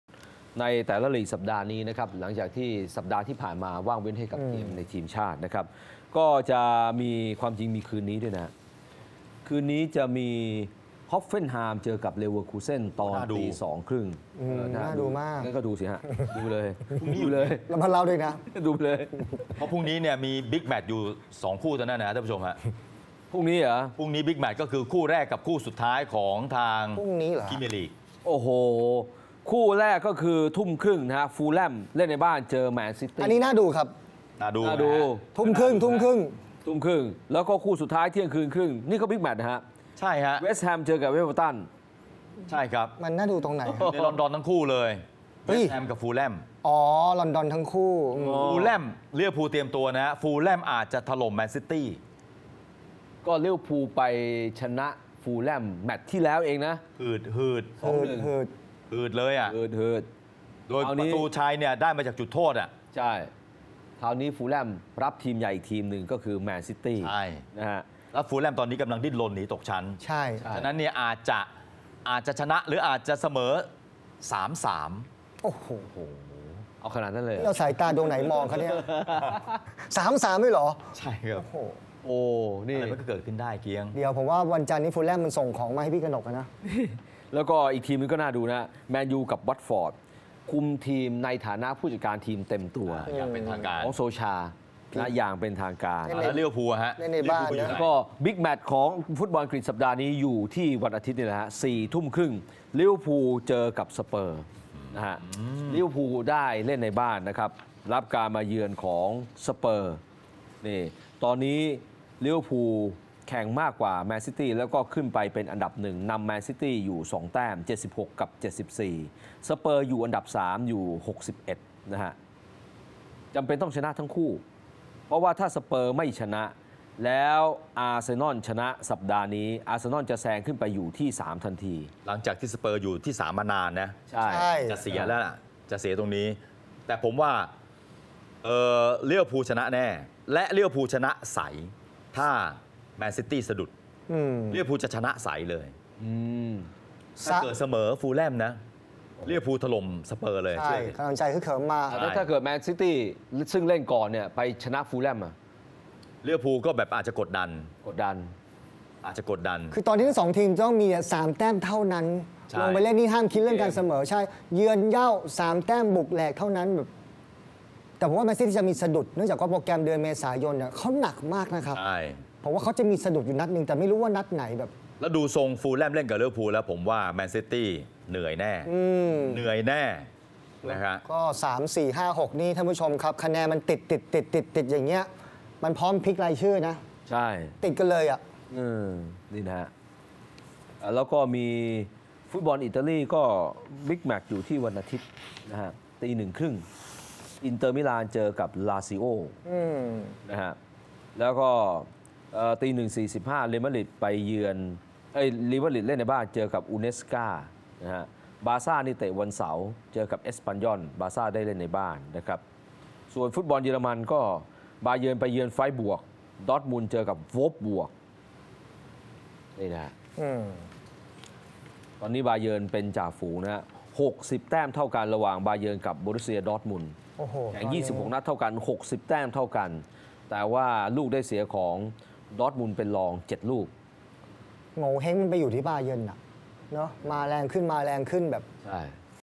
"กนก ธีระ นันทขว้าง" วิเคราะห์บอล 4 ลีกใหญ่ยุโรป สุดสัปดาห์นี้...ใครวิน ใครวืด?
ติดตามข่าวสารวงการกีฬาได้ที่ Facebook : NationTv Sport News